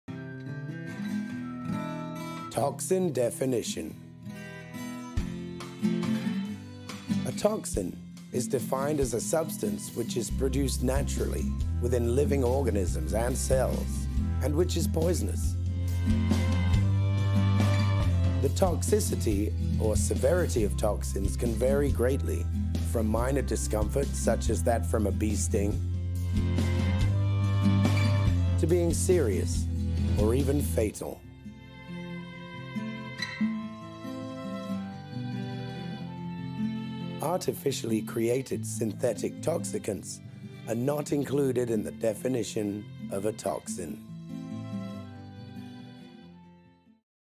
Toxin Definition
A toxin is defined as a substance which is produced naturally within living organisms and cells and which is poisonous.